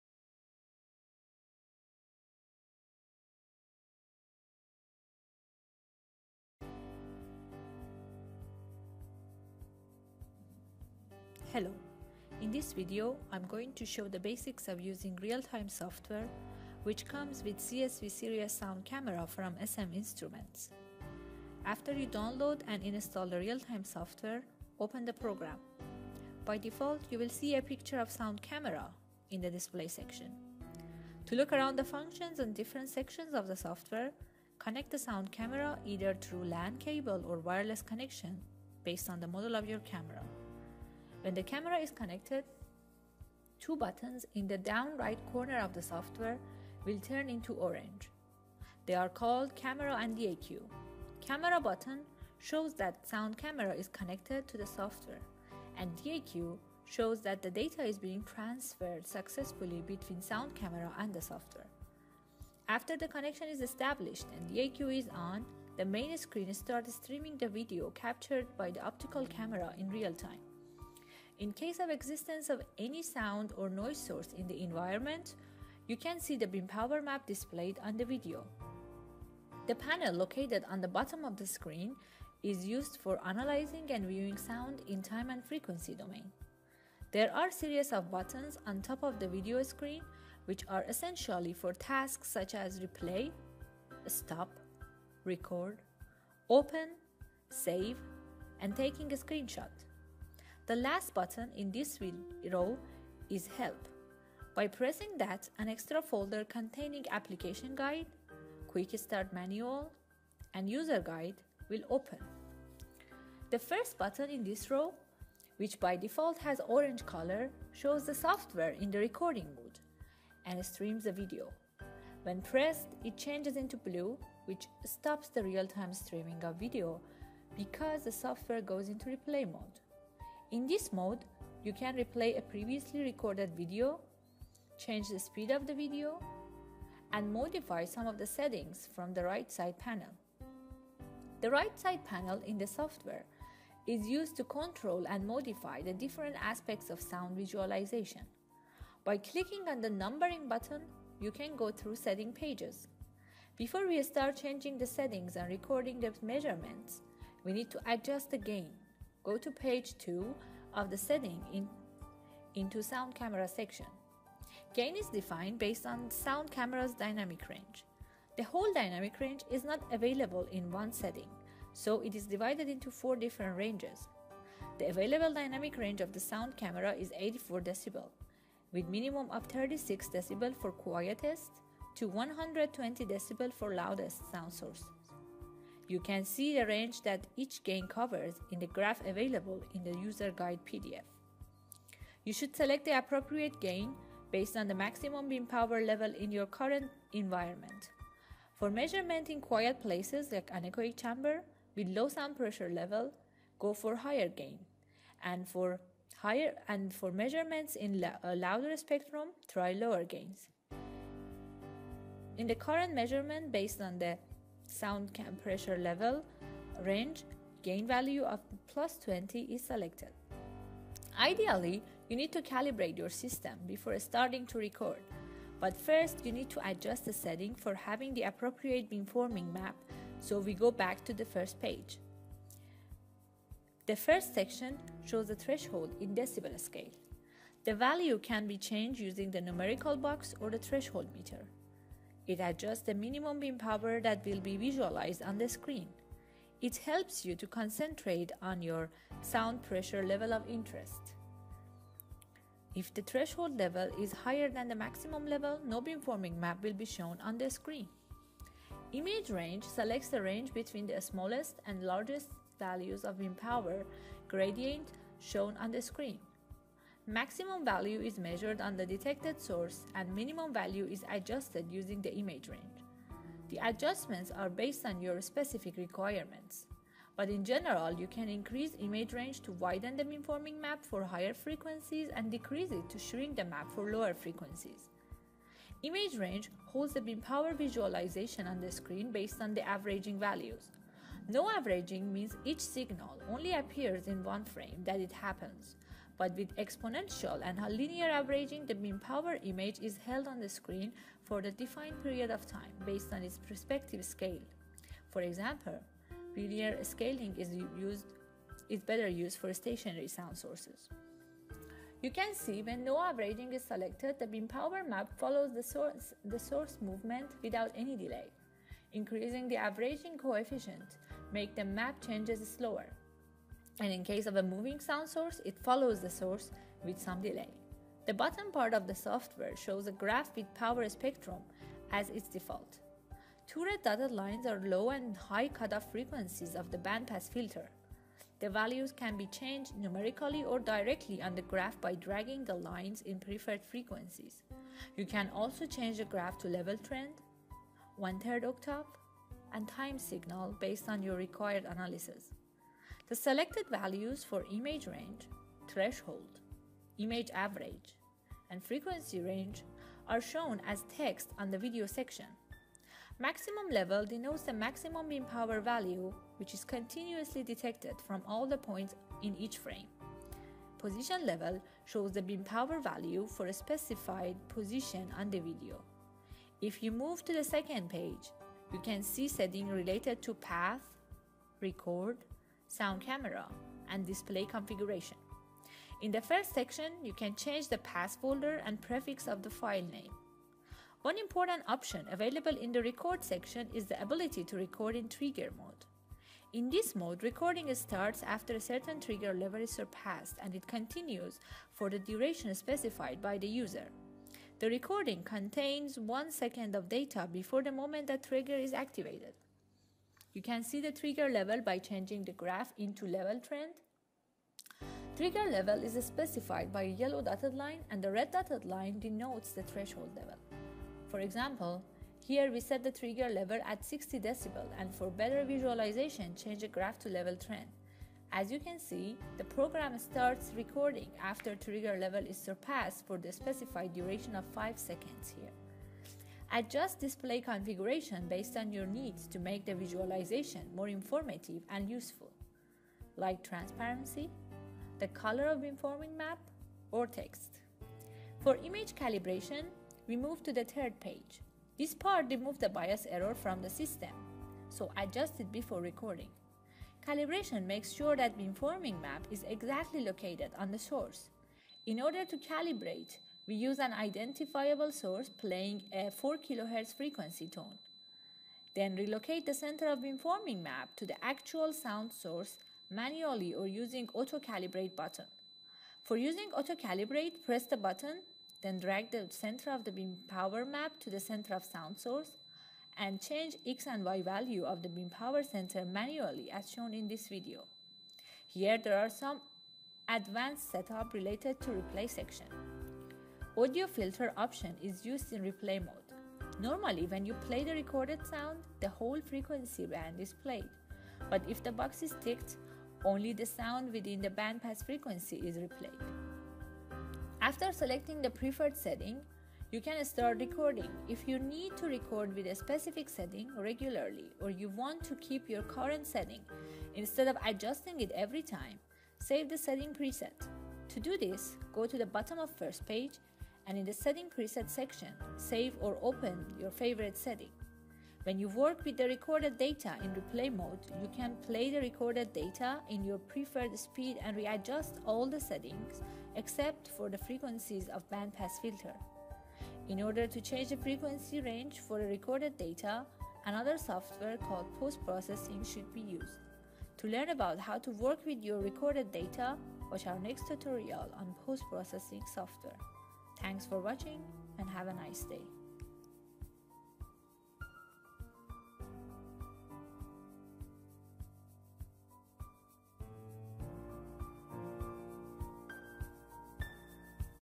[SeeSV-S205] Tutorial Video for SeeSV Real-time Software
The tutorial video helps a customer to use a sound camera, SeeSV-S205/S205W.